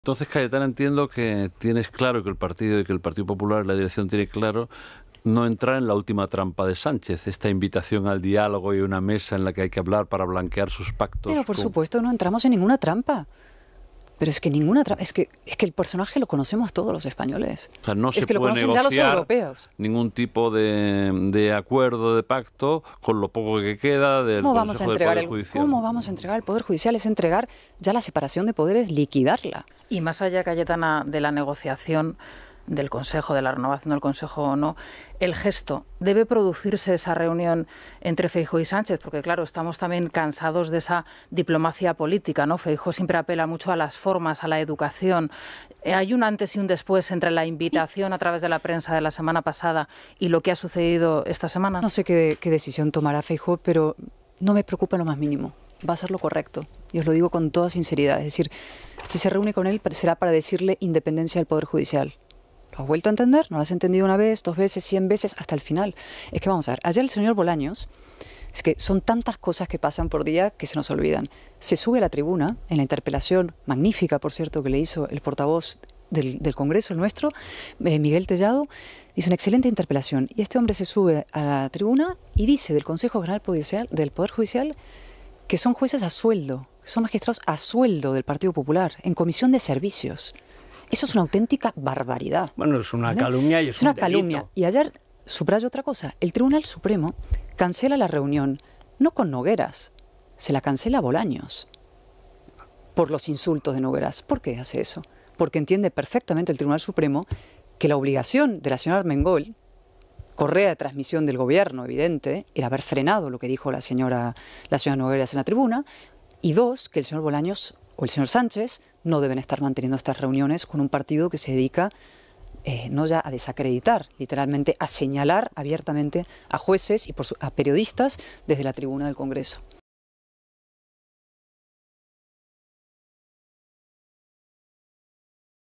Álvarez de Toledo: "Entregar el poder judicial a Sánchez es liquidar la separación de poderes"
Refiriéndose a la renovación del CGPJ, la portavoz adjunta del PP en el Congreso dice en esRadio que su partido no caerá "en ninguna trampa".